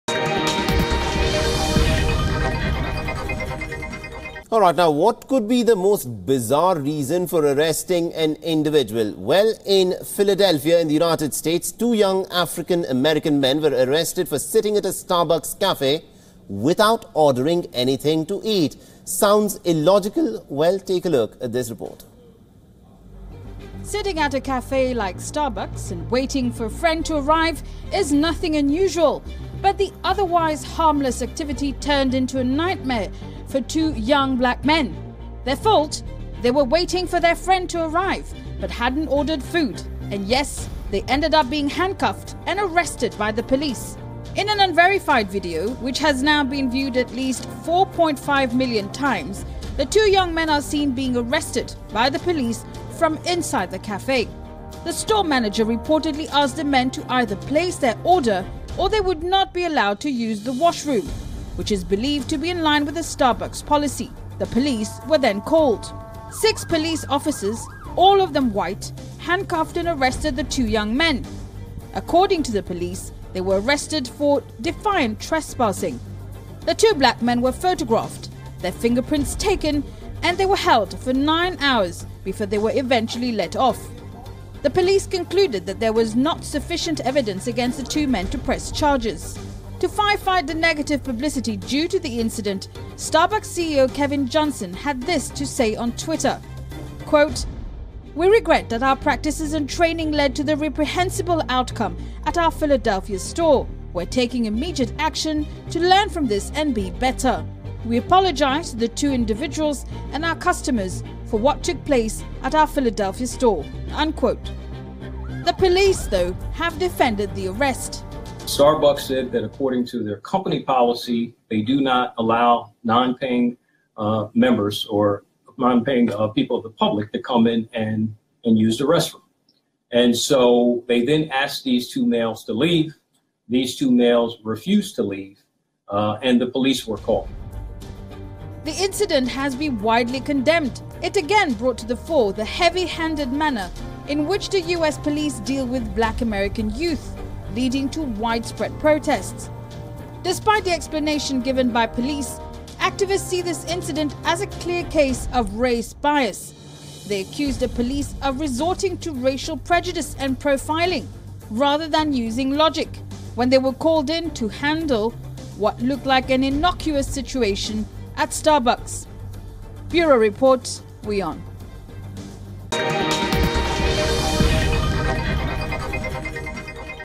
Two men arrested in US Starbucks for doing absolutely nothing
What could be the most bizarre reason for arresting an individual? Well, in Philadelphia, US 2 men were arrested for sitting at a Starbucks cafe without ordering anything to eat. Watch this video to know more.